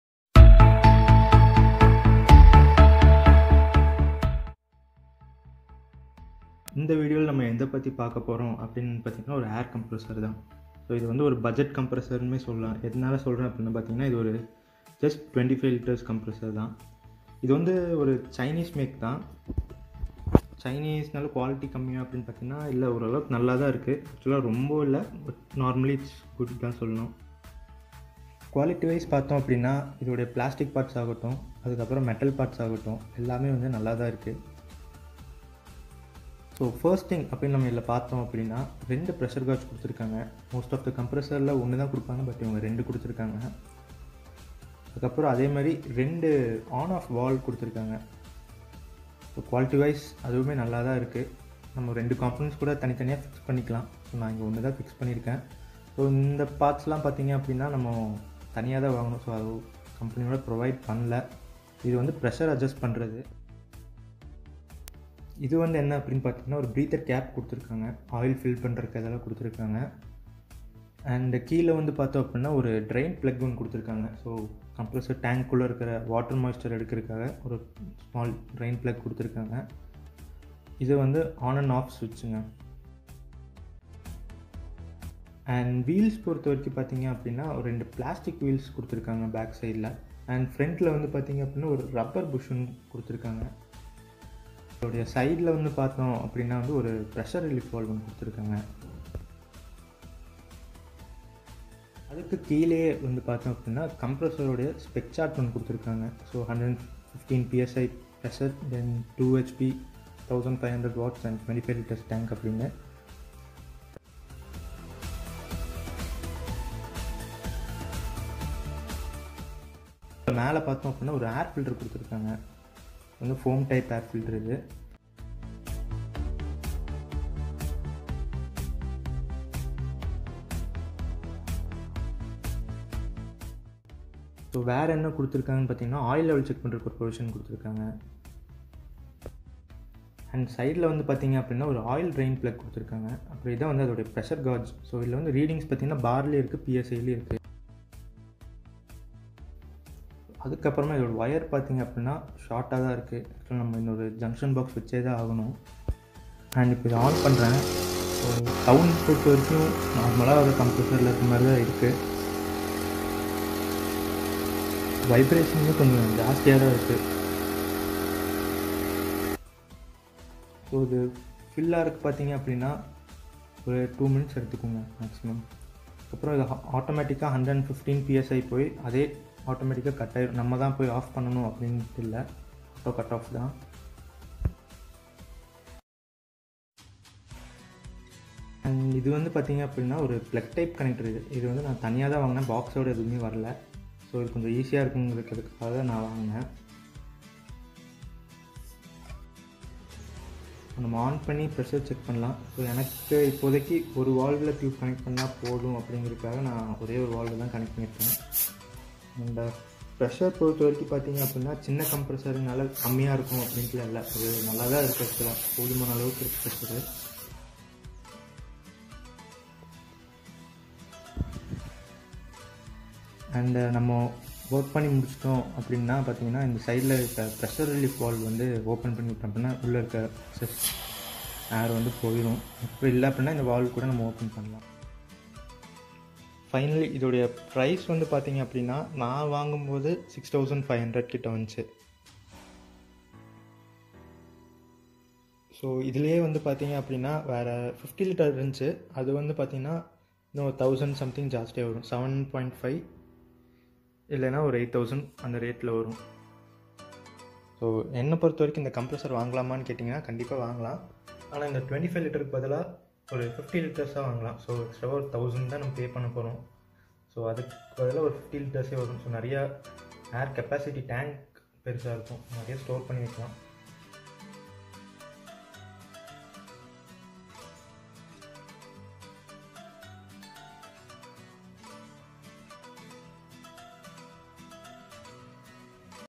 Cheap Air Compressor Review | 25L Xvnt | Tamil review For Painting
in this video we are gonna take a look on a small portable 25l air compressor
Xvnt chinese product

Music Credits